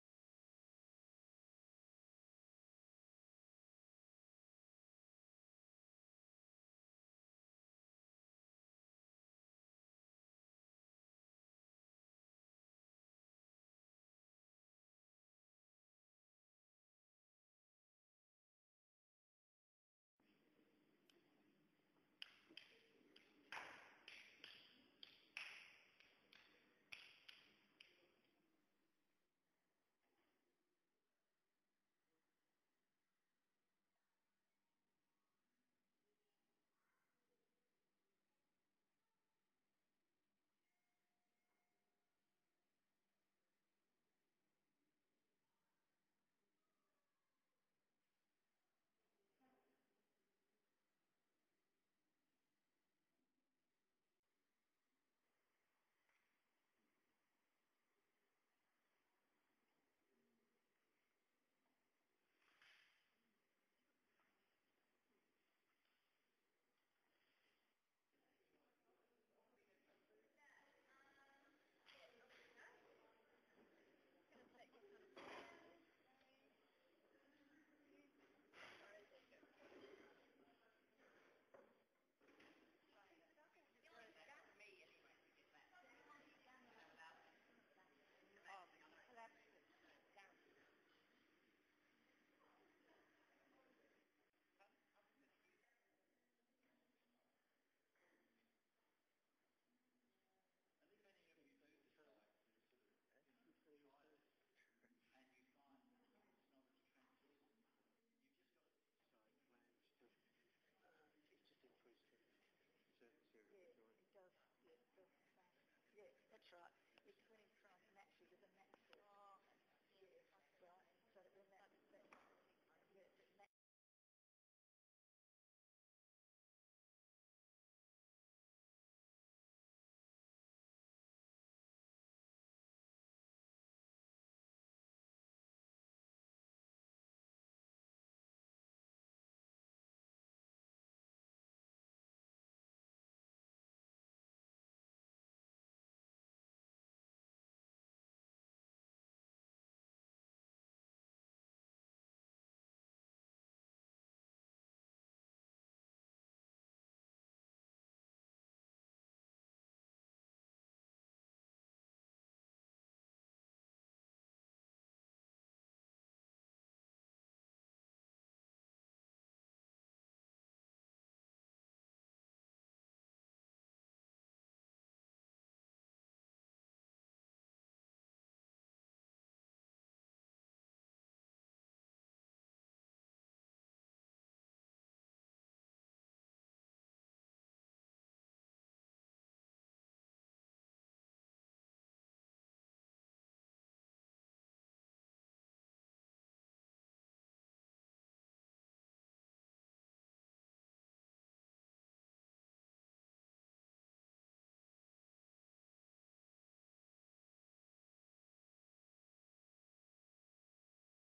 Les Blakeborough Master Potter
Les Blakeborough holds a workshop to demonstrate techniques using shellac resist to create translucent layers of calligraphy and incised patterns using Southern Ice Porcelain